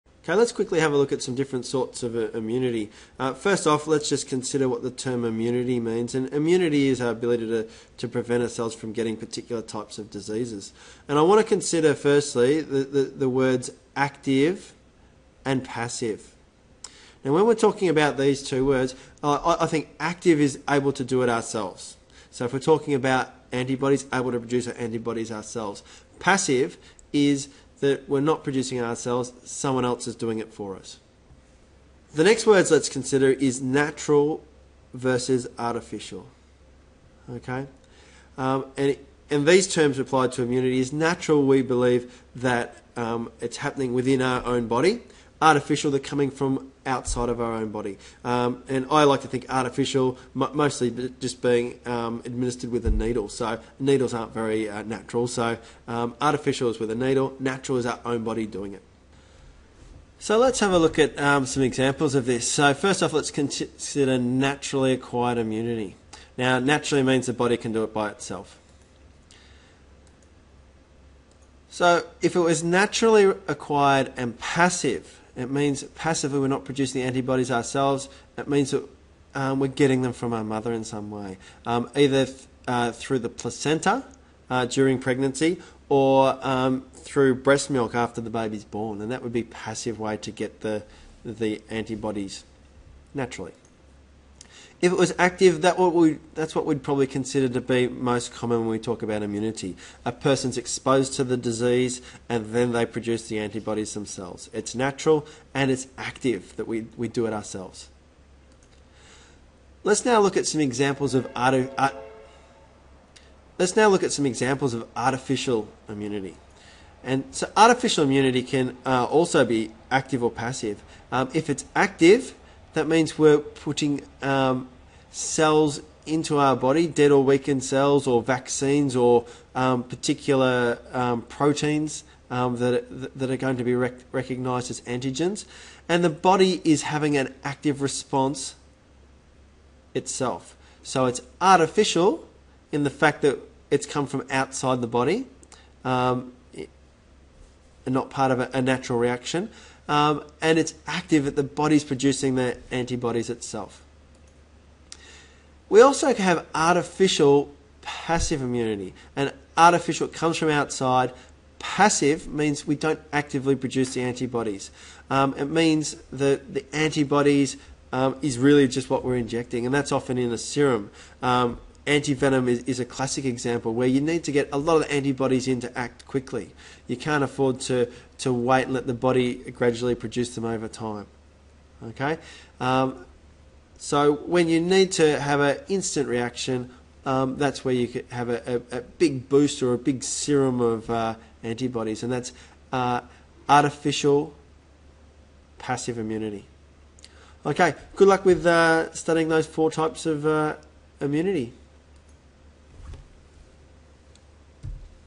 Types of Immunity
Brief outline of the types of immunity - passive-active immunity, natural-artificial immunity